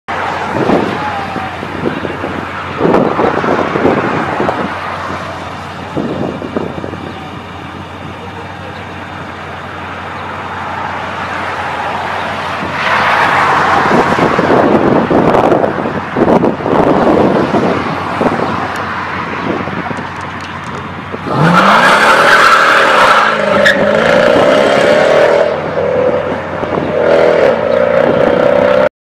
2015 jeep Srt rear wheel drive takeoff
Installed the Tazer gives me option to switch to 2/4 or 4x4